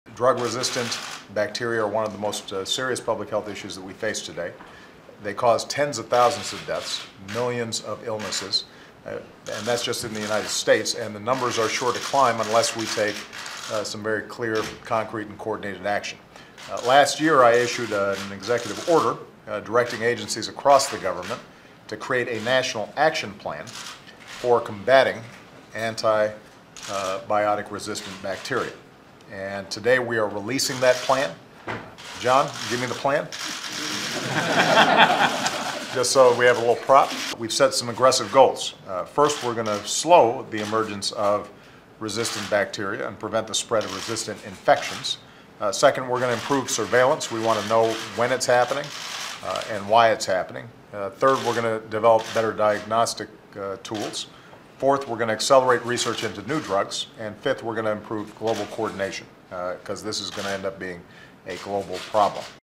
WH Plan to Fight Antibiotic-Resistant Germs
The White House on Friday announced a five-year plan to fight the threat posed by antibiotic-resistant bacteria amid fears that once-treatable germs could become deadly. (March 27)